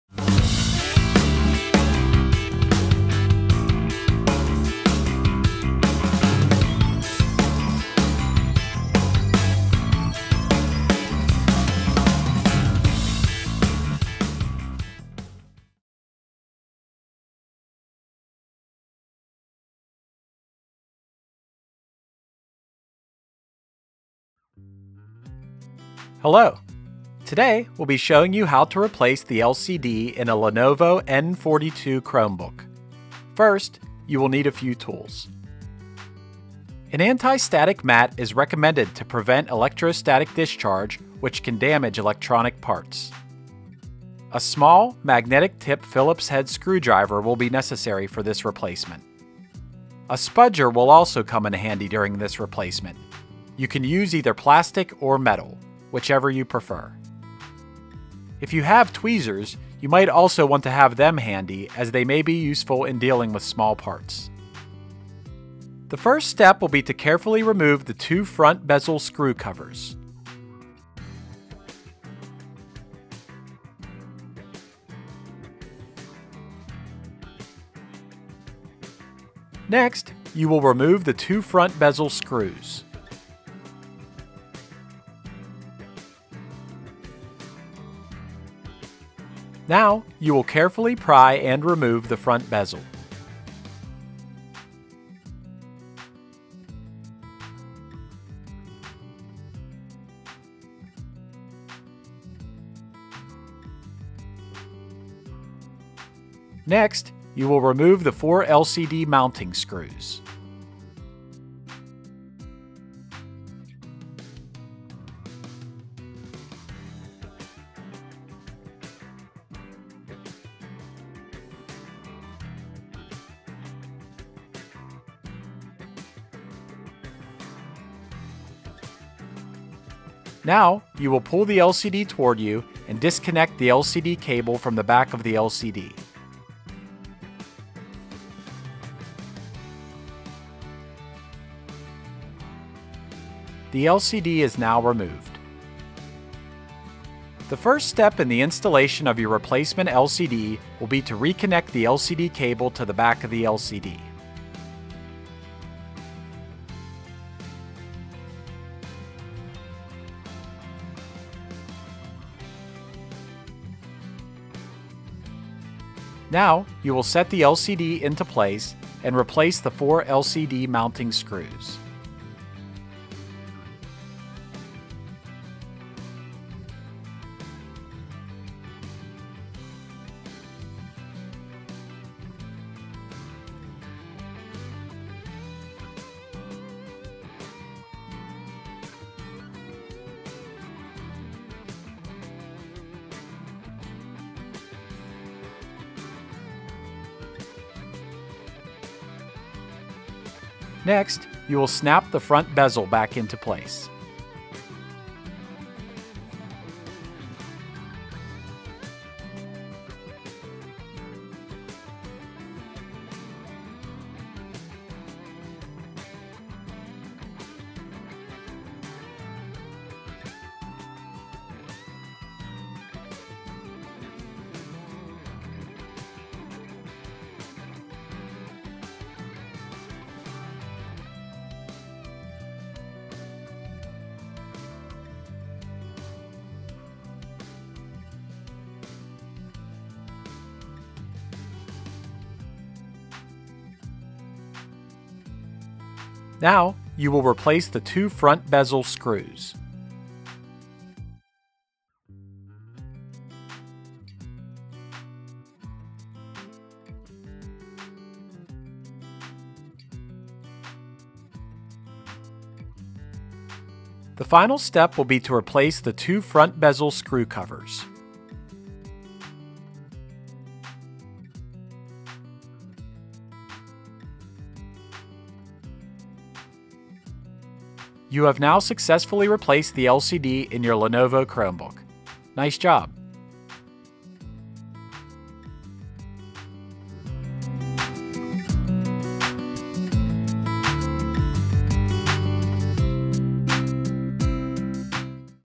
Lenovo N42 Chromebook LCD Replacement
Step-by-step tutorial on replacing the LCD on a Lenovo N42 Chromebook. For all your school's Chromebook parts and repair needs,